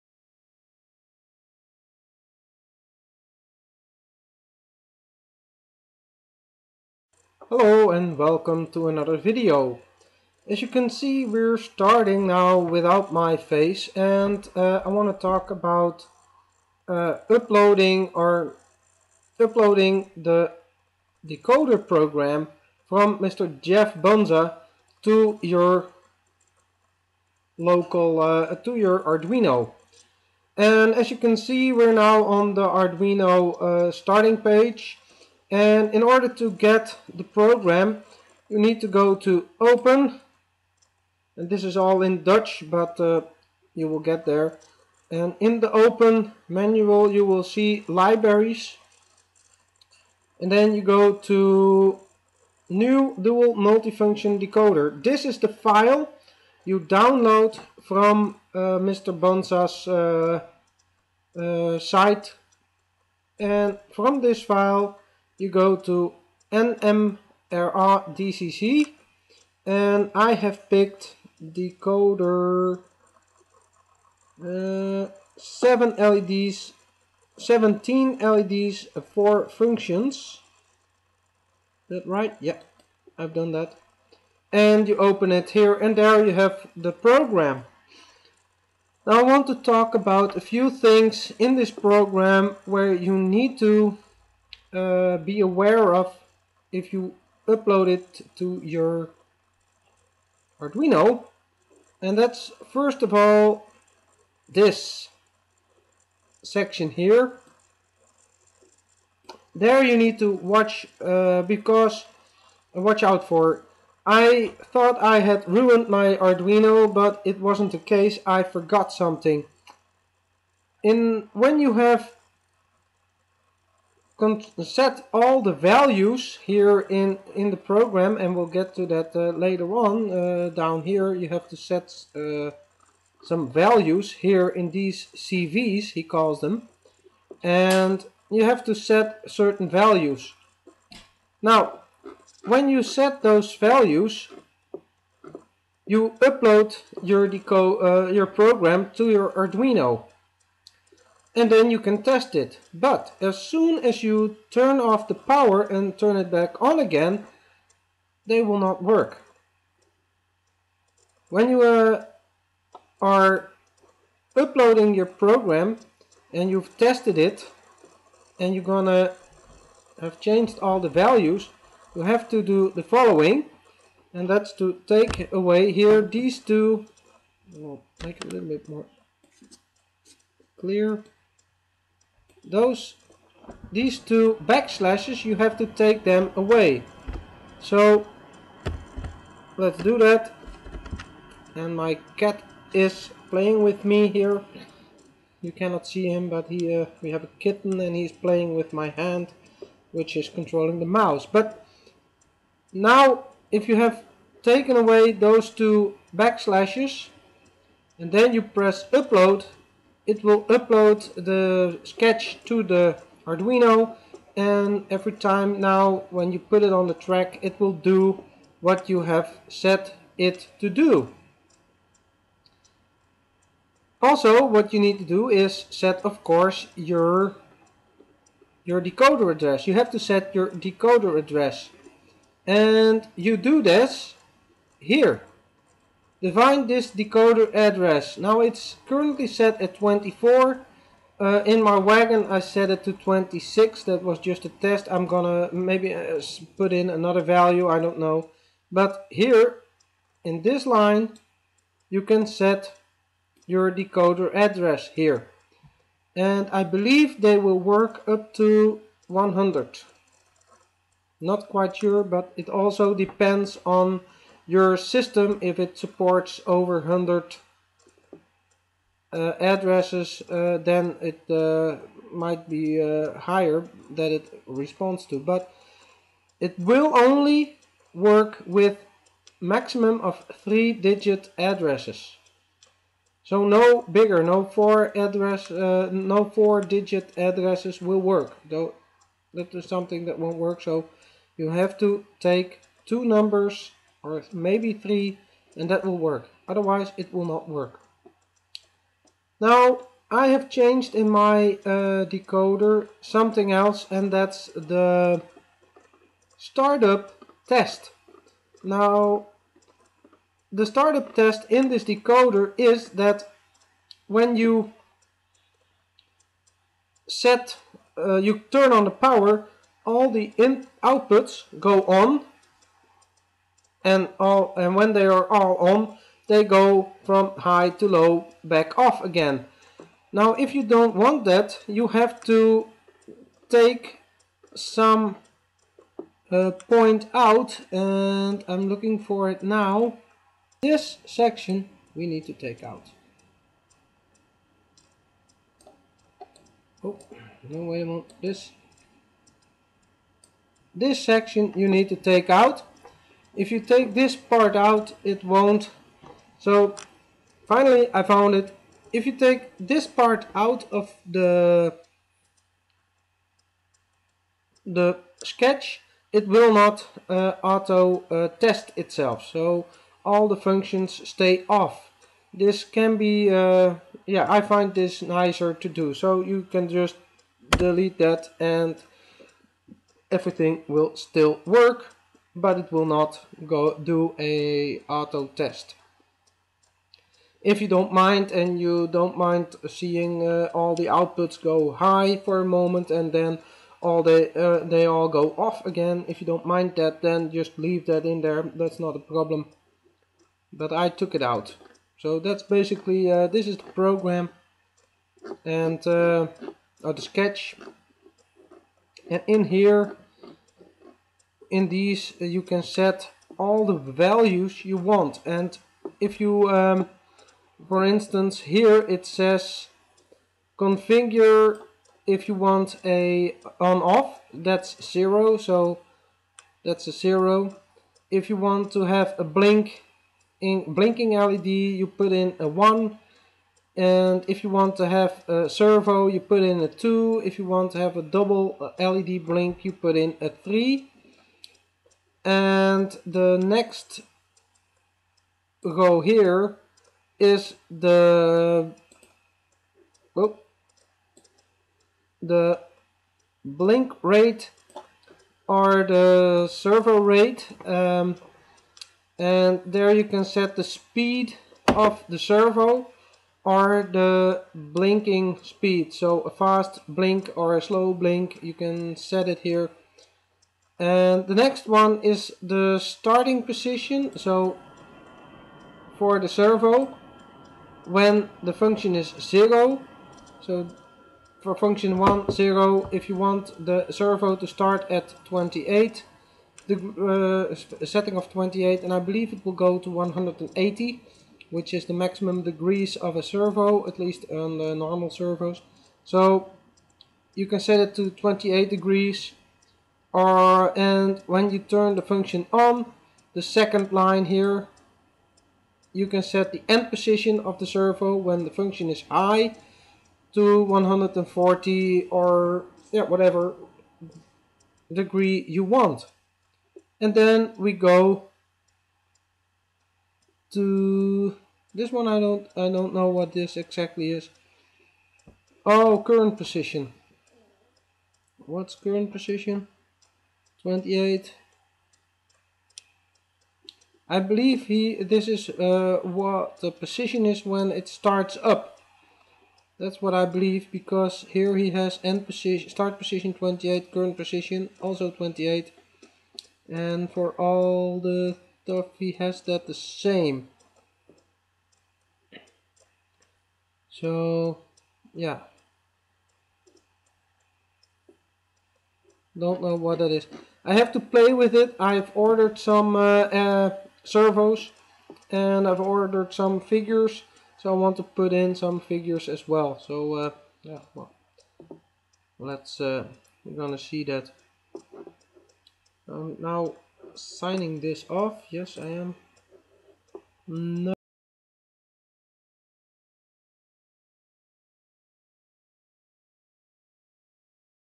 Arduino wagon decoder part 2
I am installing a Arduino dcc decoder in a passenger wagon for
controlling lights and functions .

I am using the Arduino software that was created by mister
Geoff Bunza.

In this video I take a look at the sketch and how to use and upload it.

This video is where I took a look at this sketch.

Here you can find more info.

This link is where you download the sketches and the library.